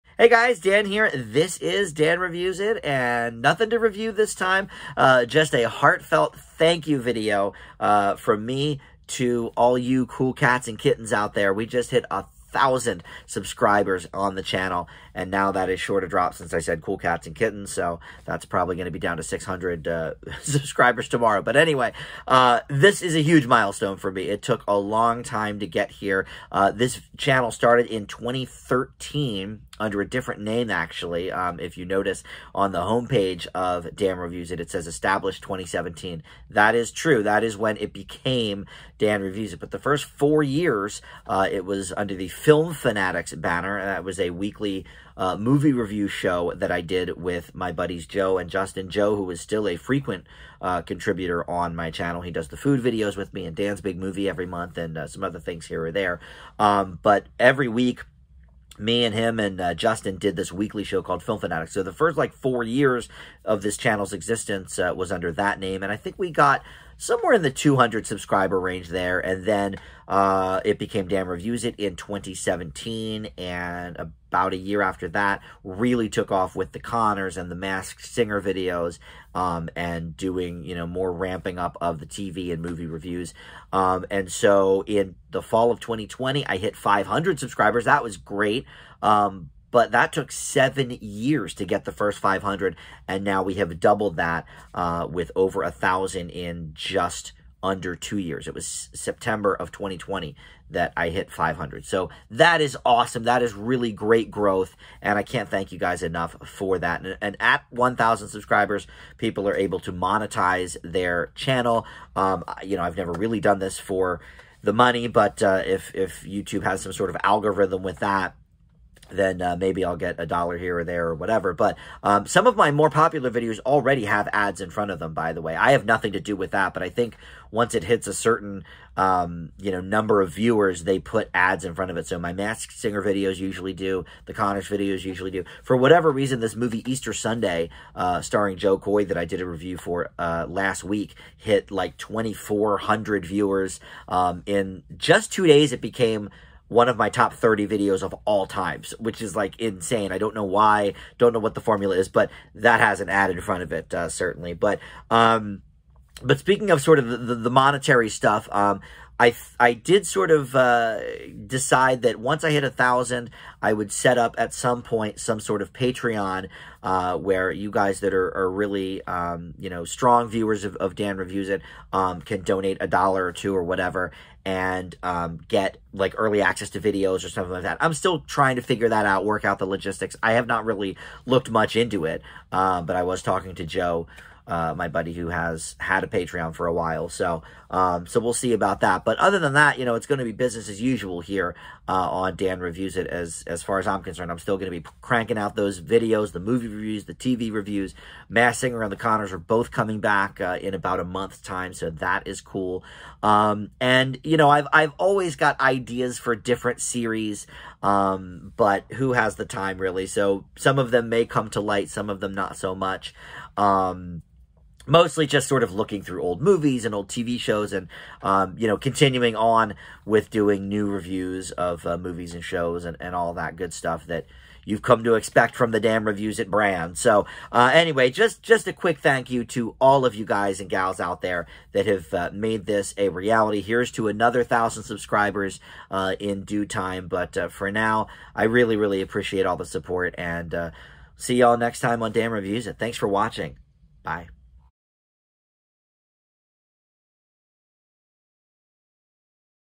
1,000 Subscribers - THANK YOU!!!
Less than 2 years after hitting 500 subscribers, I have hit 1,000 subs and it's all thanks to you!!